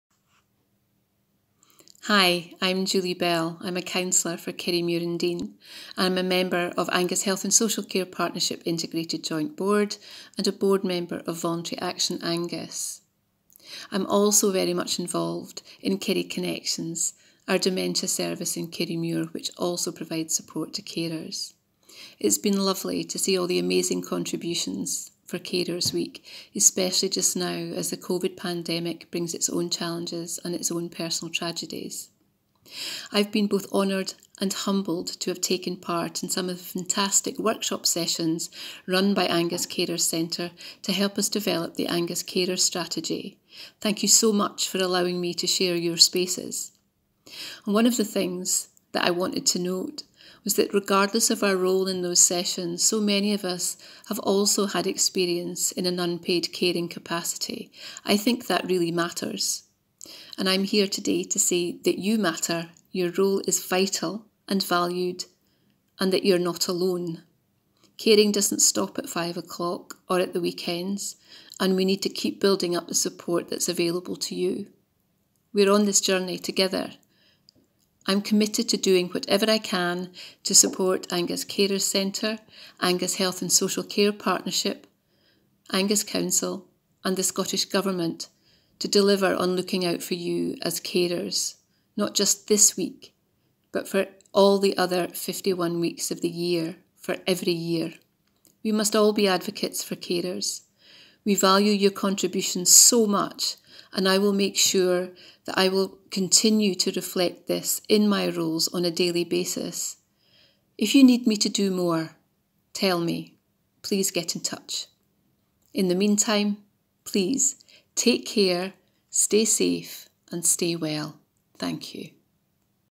A message about Carers Week 2020 from Julie Bell - Angus Councillor
As Carers Week 2020 draws to a close, Angus Councillor, Julie Bell (@JoolsBell5), highlights the importance of MakingCaringVisible for the other 51 weeks each year.